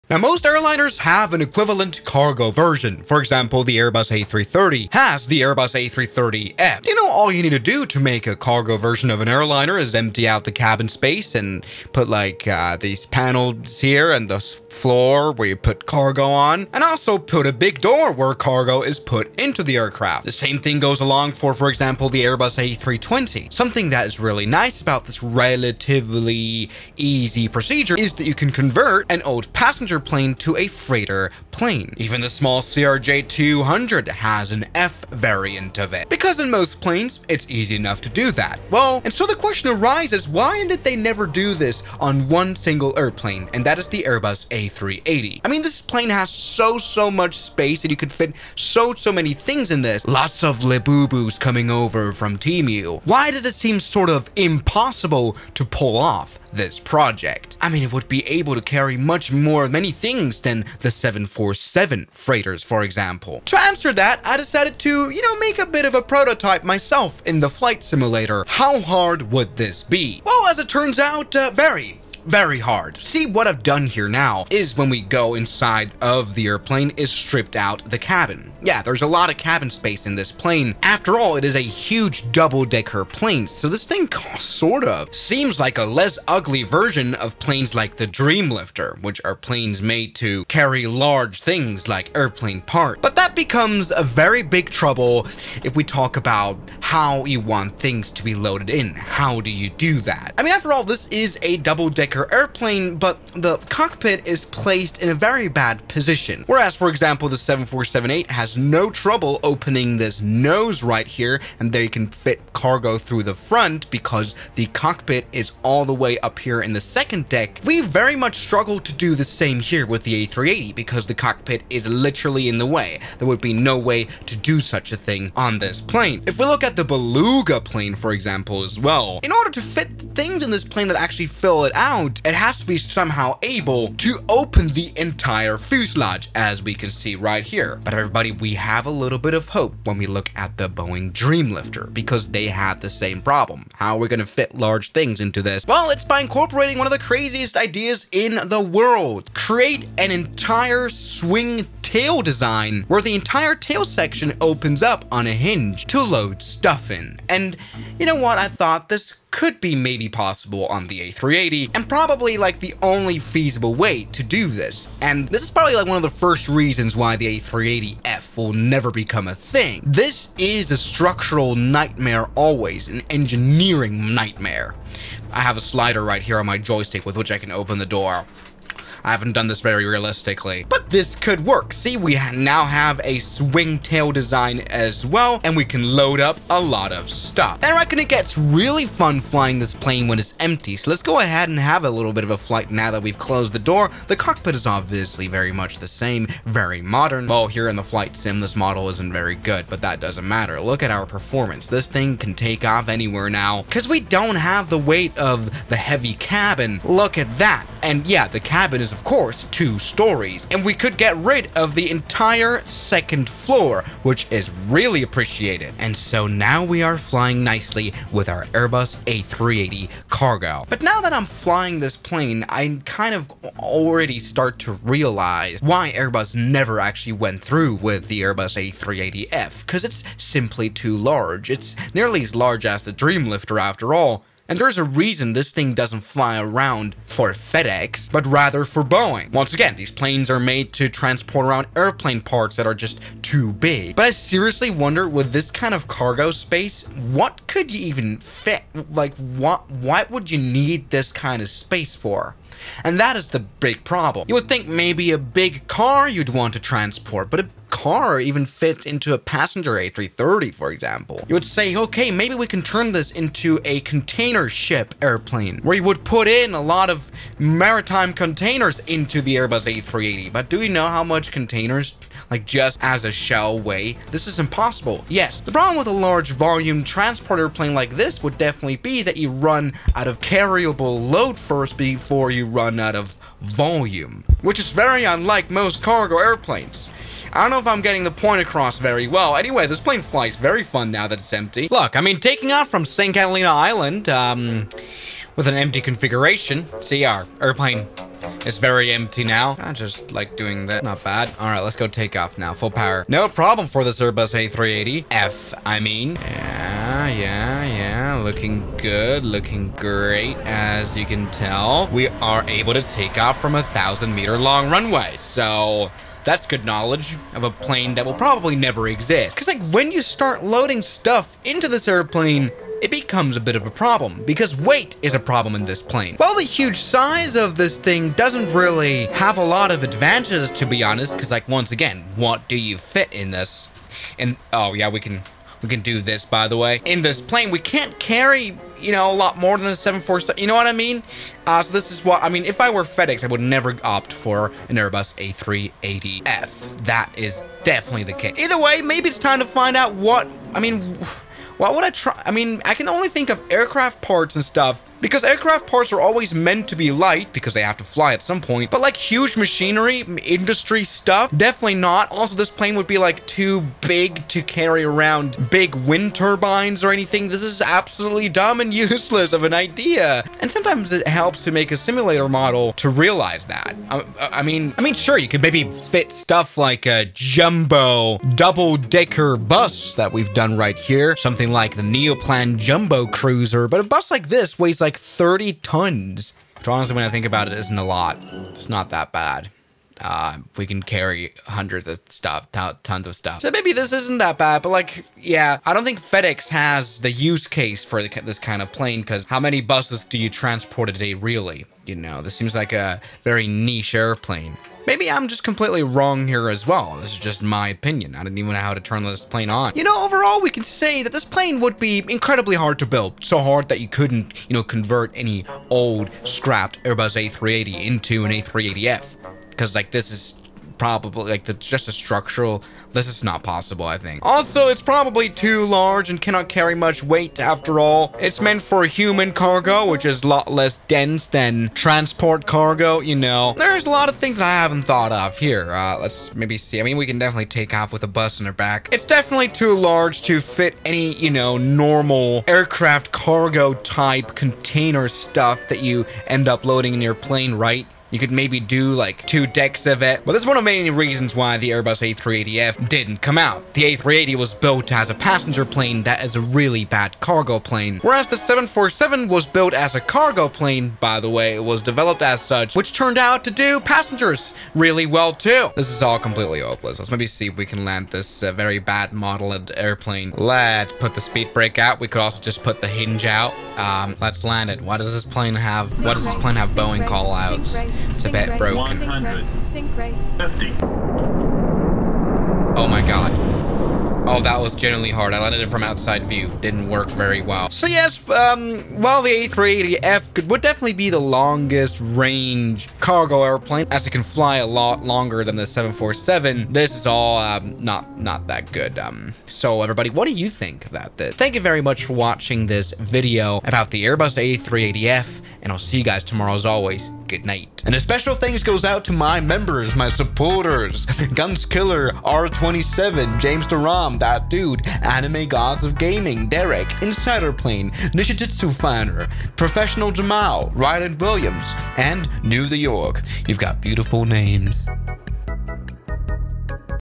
I Built An A380F Because Airbus Didn't
Today's Video: Why Airbus Never Made The A380F

Master Aviation Communication with SayIntentions.AI, my new partner!
Train real ATC radio calls, get feedback, and build confidence fast.

©Swiss001 2024